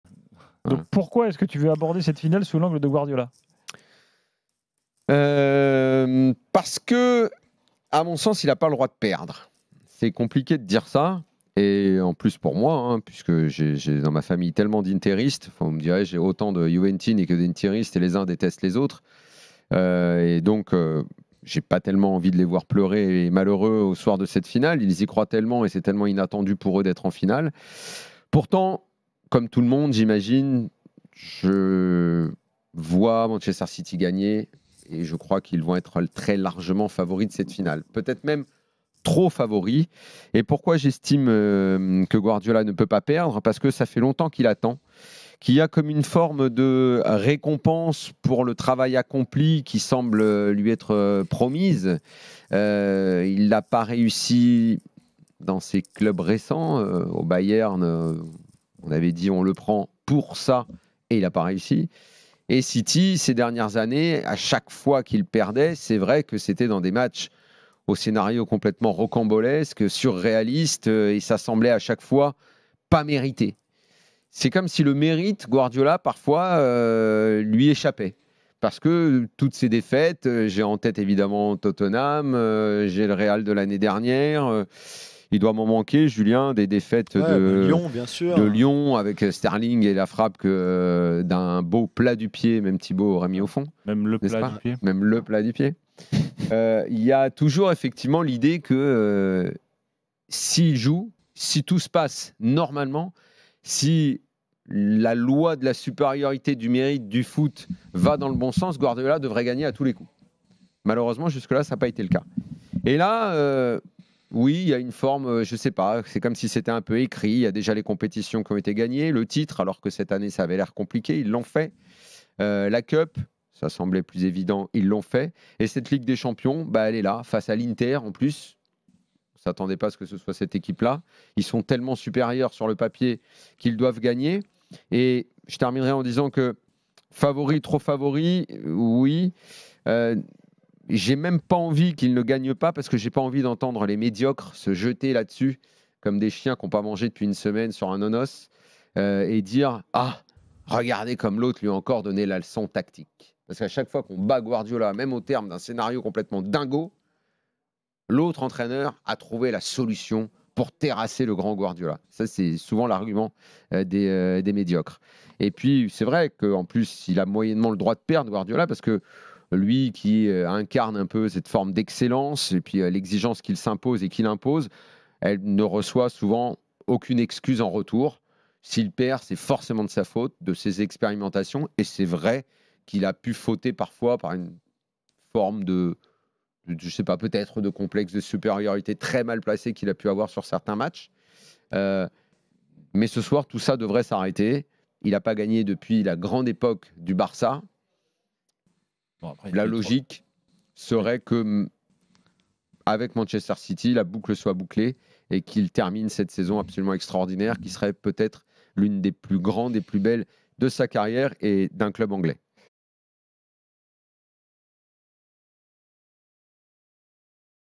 Man. City-Inter Milan : "Guardiola n'a pas le droit de perdre" selon Riolo
Pep Guardiola et Manchester City vainqueurs de la Ligue des Champions, c'est comme une évidence pour Daniel Riolo.